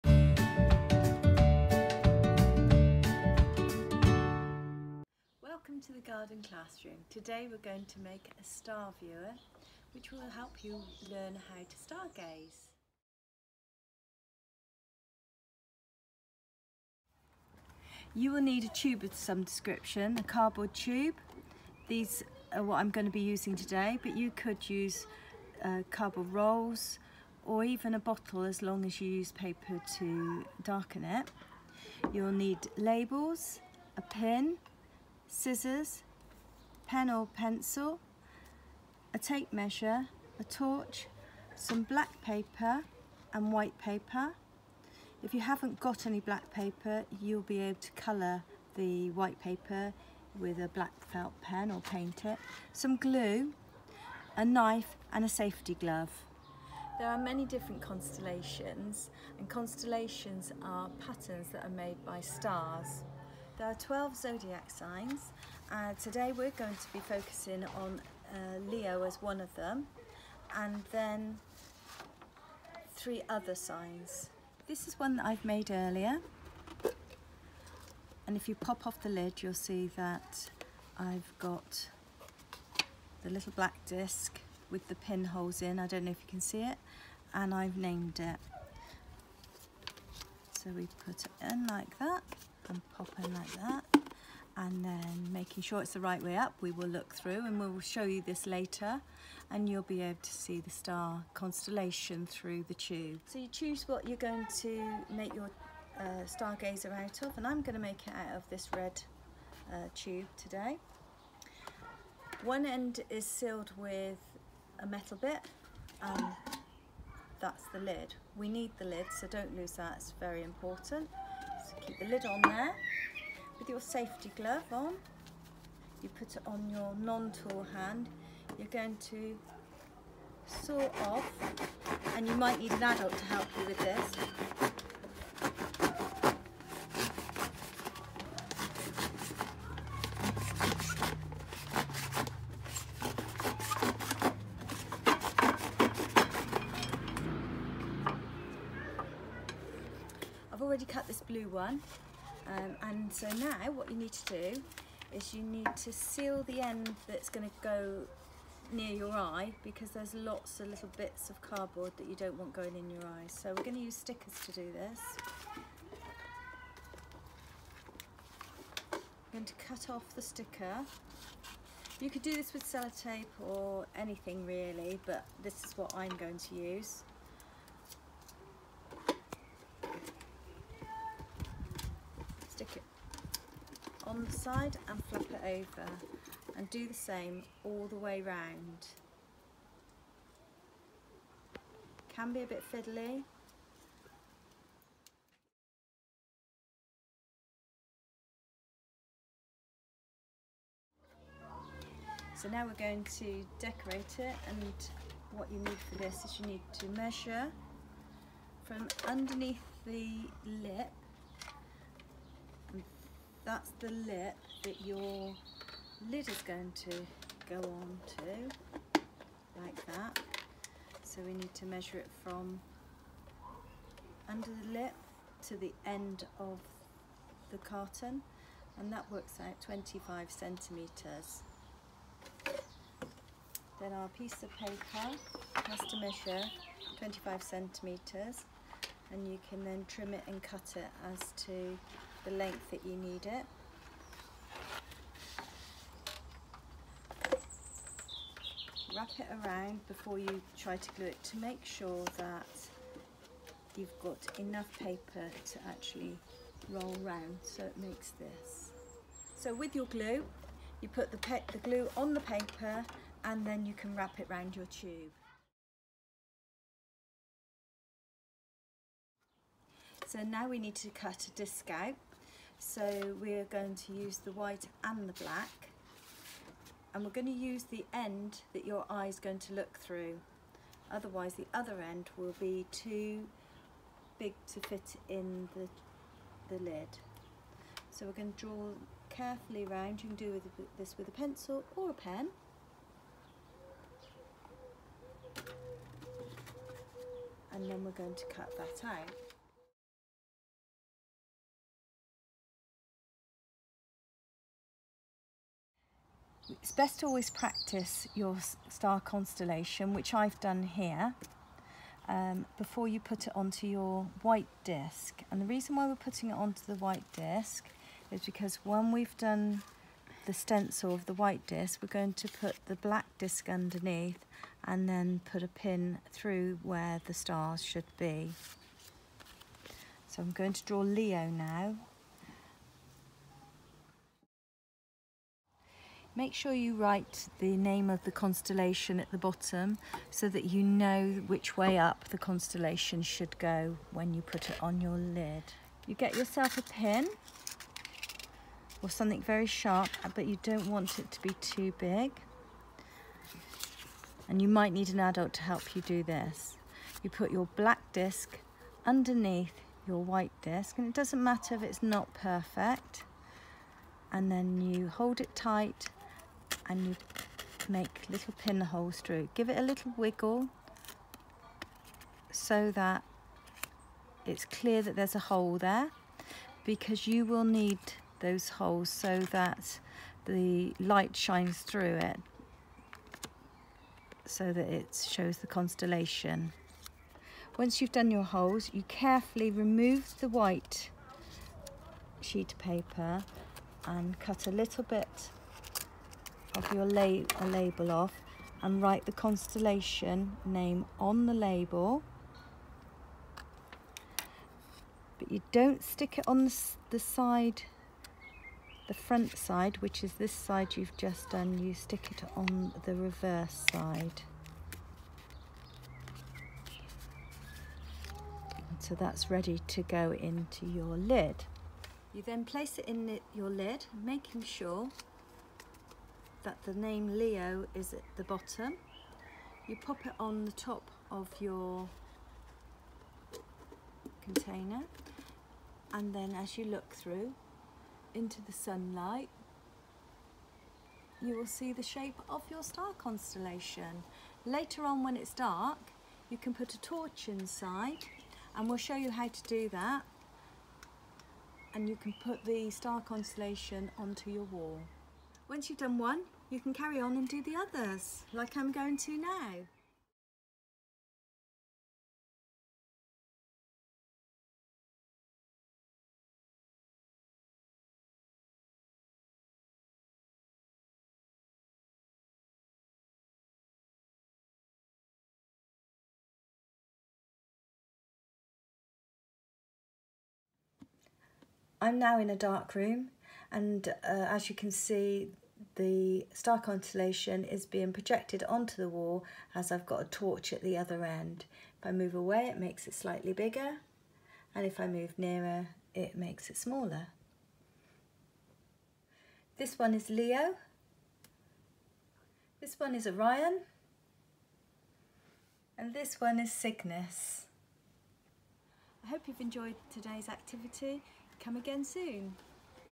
Making a Star Viewer and four constellations
If you have ever looked up into the night sky and wondered what the names of the stars are or what the constellation  are called then this activity will help you start your 'night sky journey'.
This activity will demonstrate  you how to make a  'Star Viewer' and four constellations.
1.Leo
2. Orion
3. Ursa Major (Big Dipper)
4. Cygnus

Hope you enjoy the activity and find a new dimension to 'your' world.